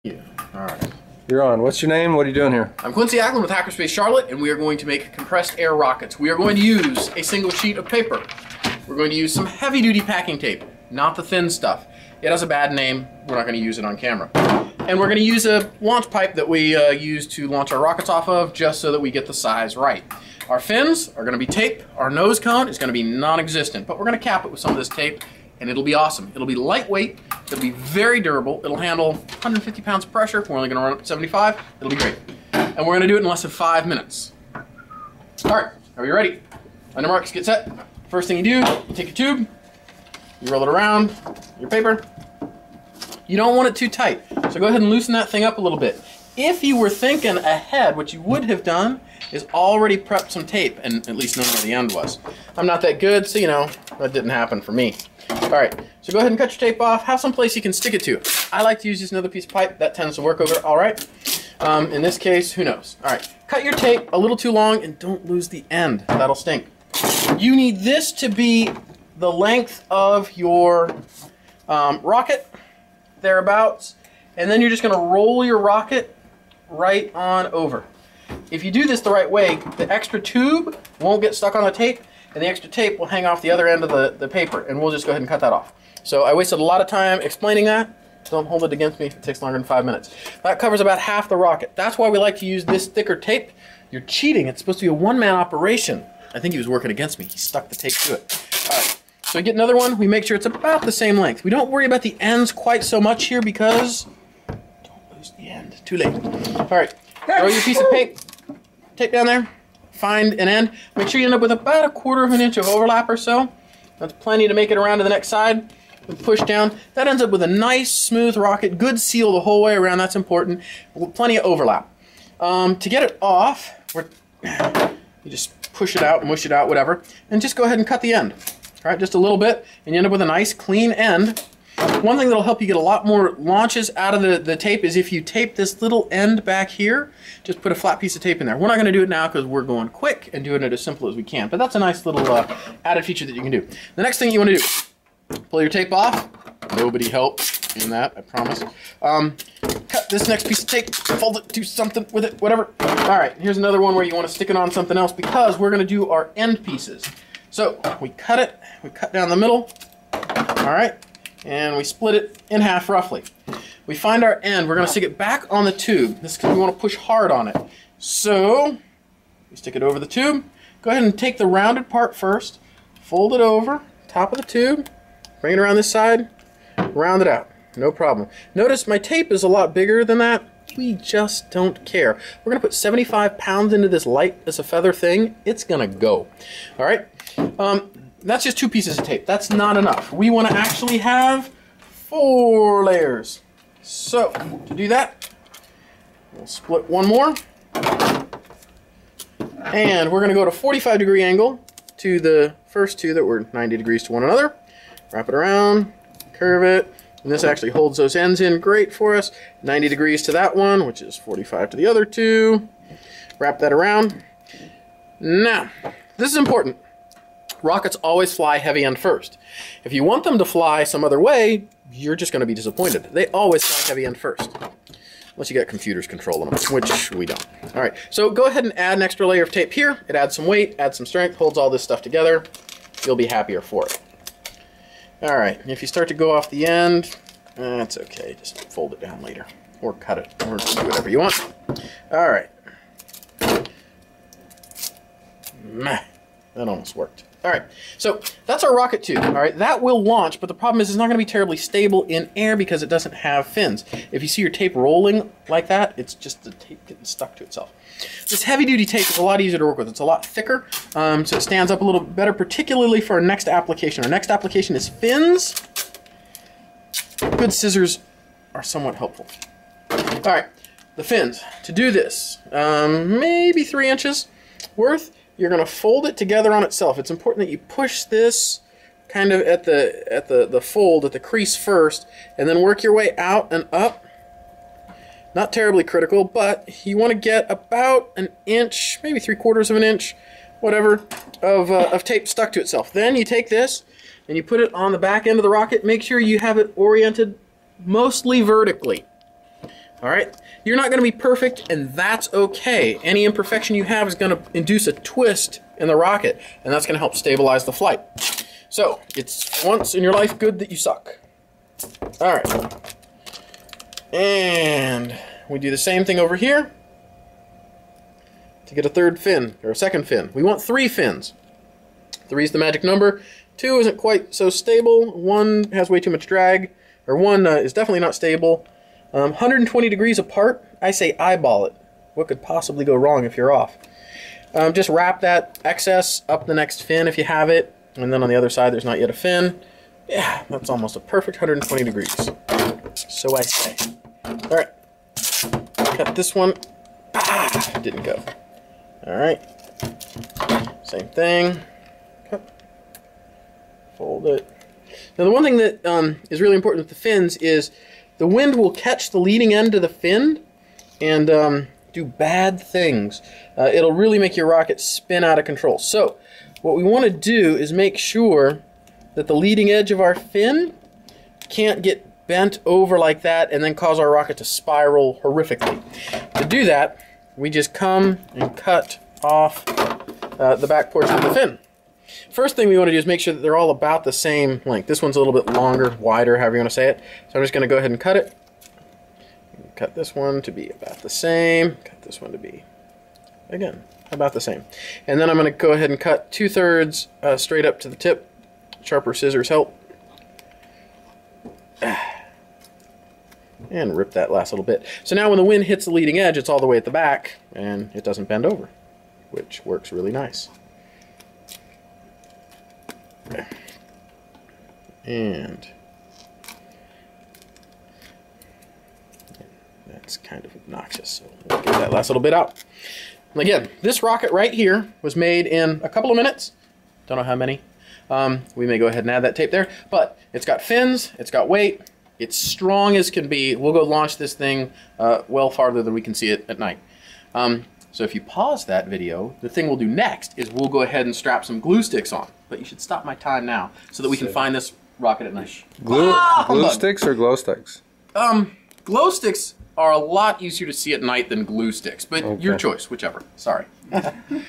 How to build a simple air rocket part I
Quincy Acklen of Hackerspace Charlotte shows basic air rocket building